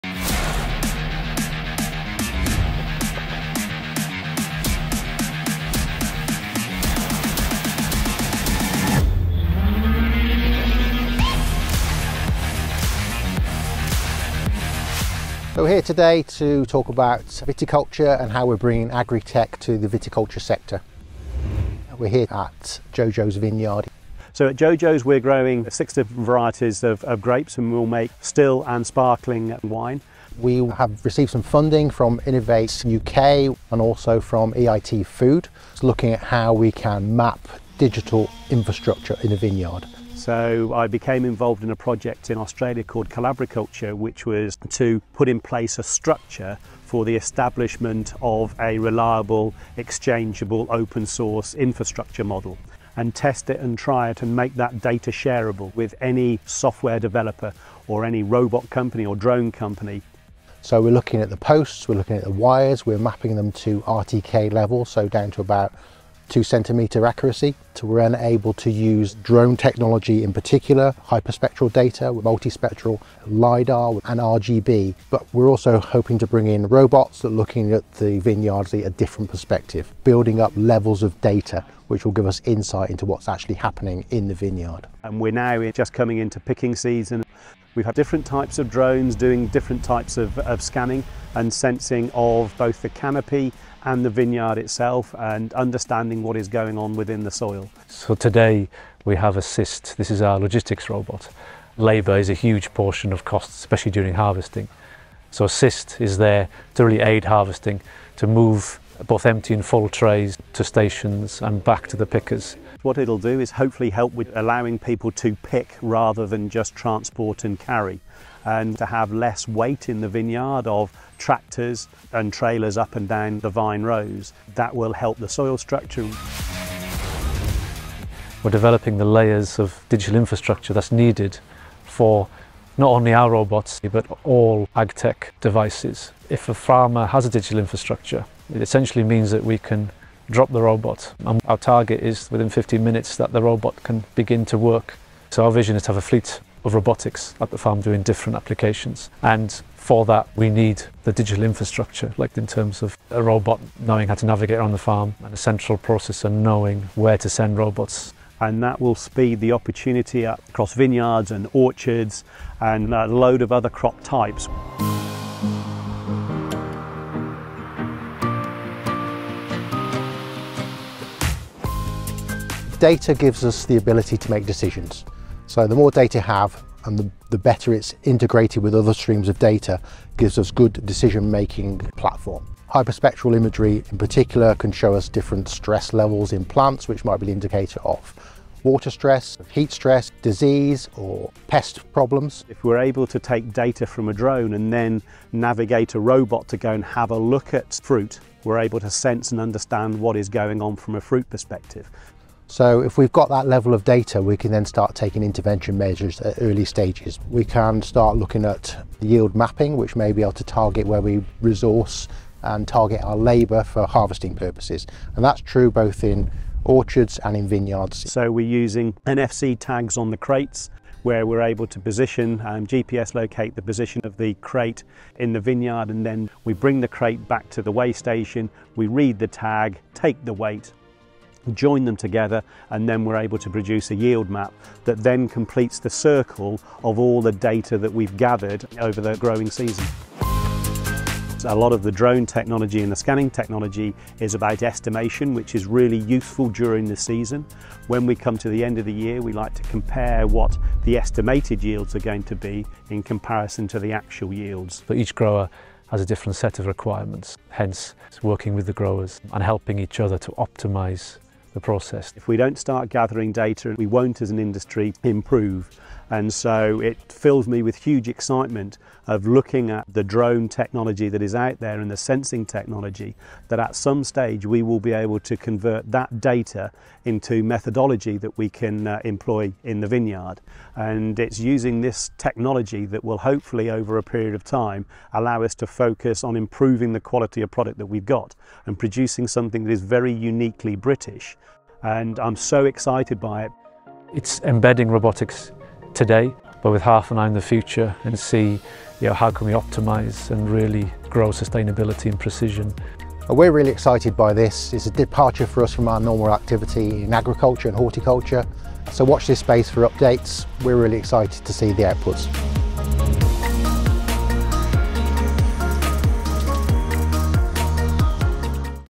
Smart Viticulture: Data and robotics project
New data and robotics projects could bring much needed time, cost and labour savings to UK vineyard producers.

Precision agriculture specialists, Agri-EPI Centre, AI-driven autonomous robotics company Antobot and vineyard owner, Ian Beecher-Jones, have embarked on two projects at JoJo’s vineyard near Henley-on-Thames to create a vineyard digital map, and on-the-ground and aerial monitoring.

The shareable digital infrastructure project – funded by Innovate UK and Defra as part of their Farm Innovation Programme Research Starter Round 2- will create the digital infrastructure of the vineyard, including rows, posts and vines to an accuracy of two centimetres using real time kinetic (RTK) surveying tools. The shareable infrastructure model, based on the Australian Collabriculture project could save producers many hours of work and cost in setting their vineyards up ready to embrace viticultural technology.

On-the-ground and aerial monitoring will be gathered by robots and drones to add a layer of data to the digital map. The robots are being developed by agriculture robot technologists, Antobot, and drones are supplied by Agri-EPI Centre. This second strand is funded by the European Institute of Innovation and Technology (EIT).

The resulting technology will be highly transferable to other row crop sectors, such as orchards and soft fruit.